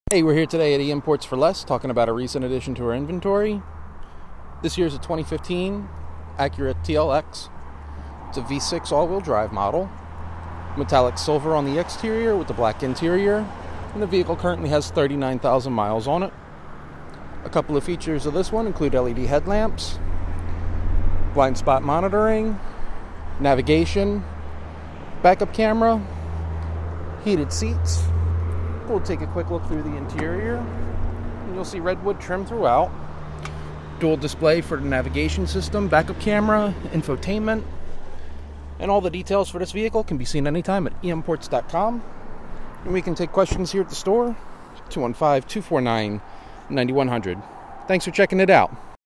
2015 Acura TLX V6 SH-AWD Sedan for sale at eimports4Less
2015 Acura TLX V6 SH-AWD Sedan for sale at eimports4Less in Perkasie, Pa.  Silver metallic over ebony leather, with 39,000 miles. Navigation, back up camera, LED headlights, blind spot monitoring, adaptive cruise control, heated seats,  keyless entry and ignition, fog lights, moon roof, power seats,  park distance control, dual zone climate, HomeLink, XM satellite radio, Bluetooth, touchscreen display and more.